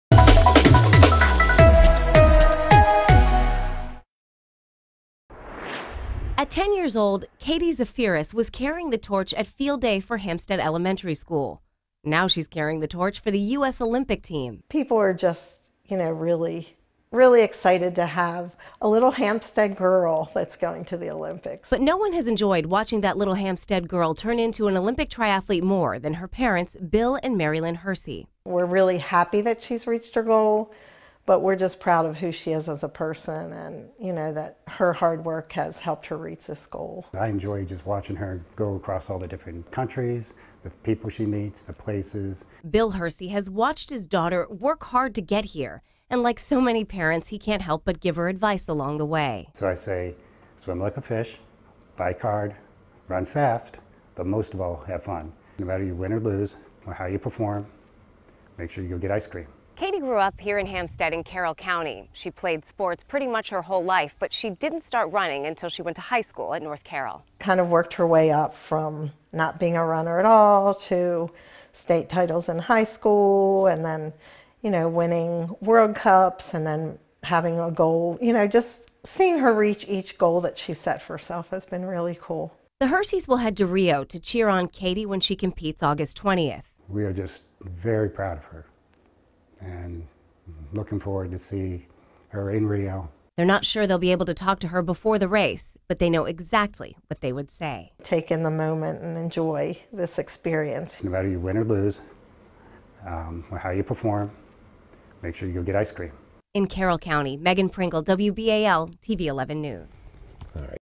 Hampstead woman ready for triathlon in Rio
When she was 10 years old, Katie Zaferes carried the torch at field day at Hampstead Elementary School in Carroll County. No one has enjoyed watching that little Hampstead girl turn into an Olympic triathlete, more than Zaferes’ parents.